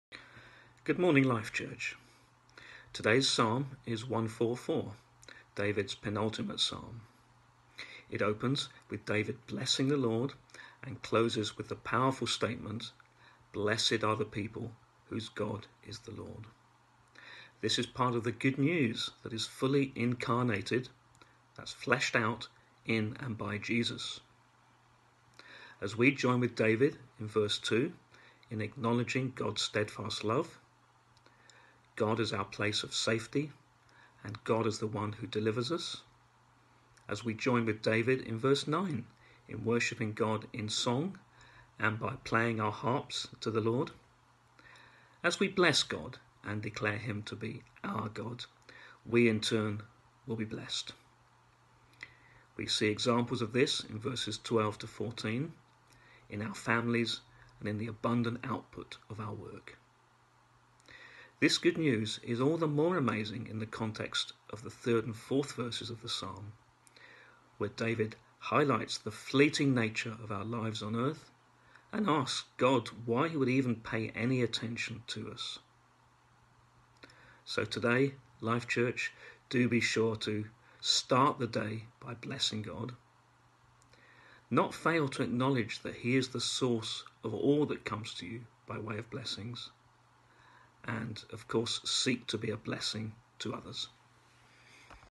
Daily Devotional - Psalm 144
David Rolles talks about King David's penultimate psalm.